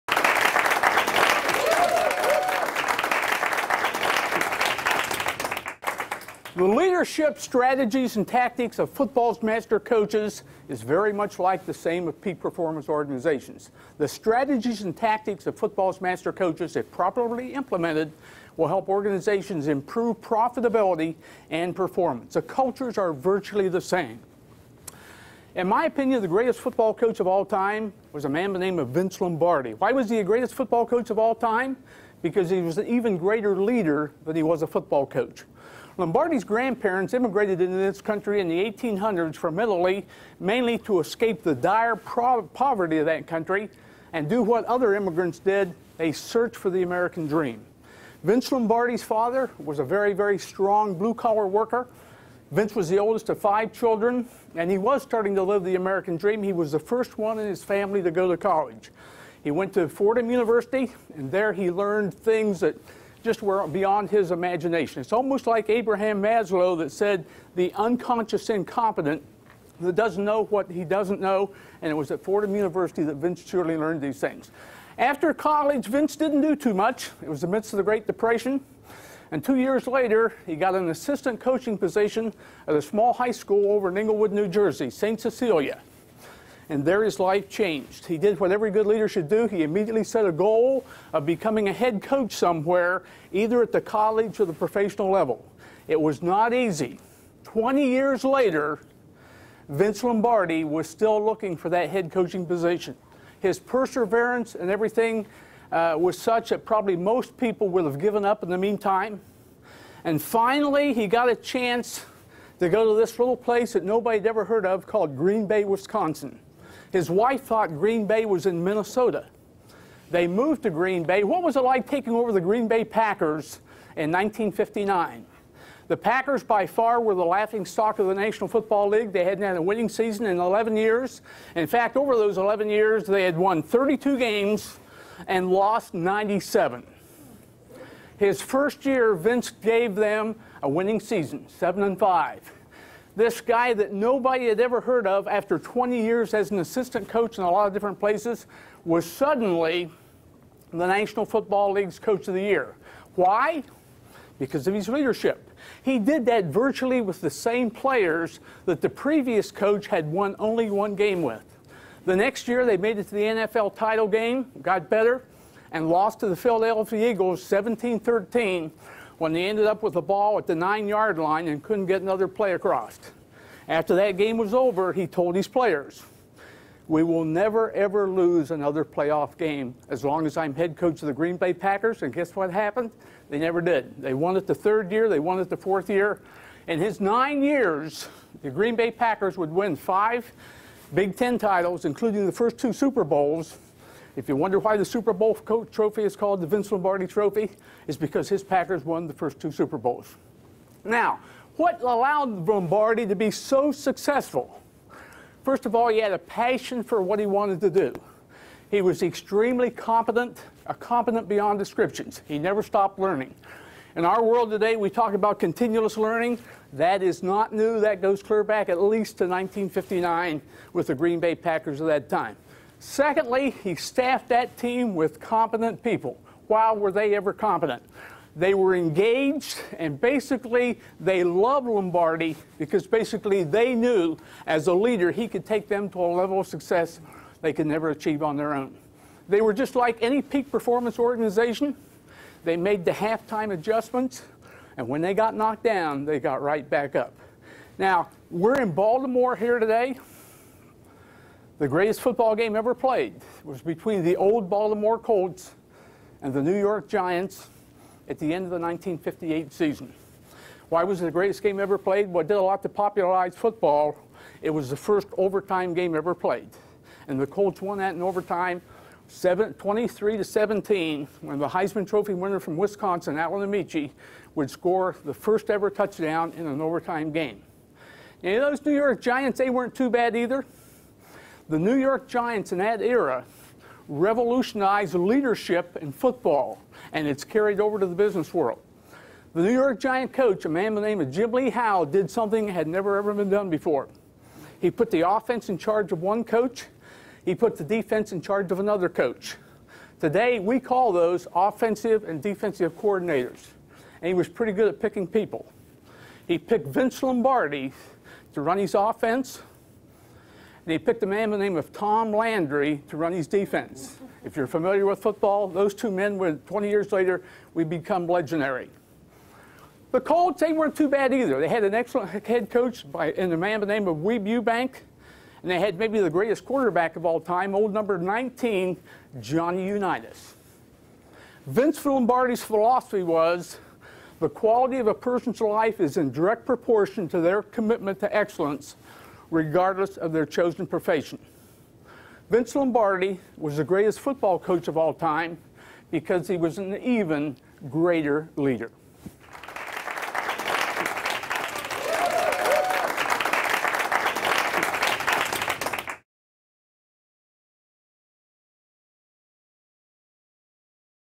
BLI Live: Jack Park talks about Strategies and Tactics of Footballs Master Coaches
Strategies and Tactics of Footballs Master Coaches

A BLI Live Talk by Jack Park

There are multiple common threads between championship football teams and high performance organizations. Many of the high-impact strategies of America's most successful football coaches can be adapted by CPAs to improve an organization's performance. Successes on the gridiron and in the boardroom are often enhanced by similar tactics. Coach Vince Lombardi's leadership model for crafting high performance organizations has become legendary. Business leaders have found Coach Bill Walsh's "Find the Winning Edge" system to be very effective in helping them grow their organizations. This CPE program will focus less on what coaches like Lombardi and Walsh accomplished and more on how they, and other master coaches, led their people to extraordinary levels of success.